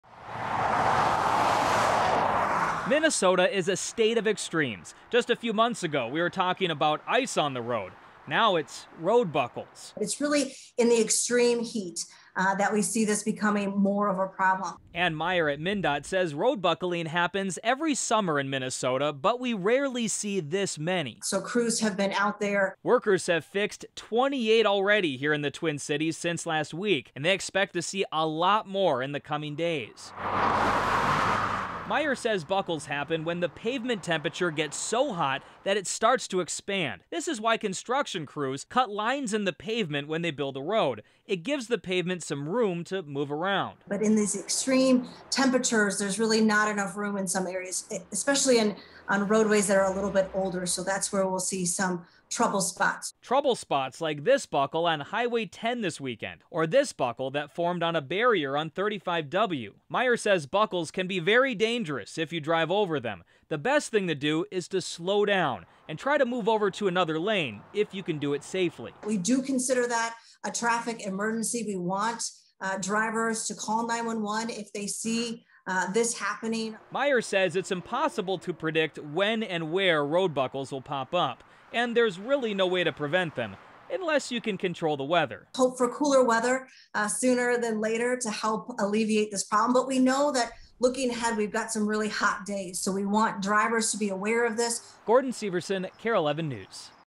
MnDOT crews fix 28 road buckles due to extreme heat, even more expected later this week
MnDOT is asking drivers to be on the look out for road buckles as temps stay in the 90s this week.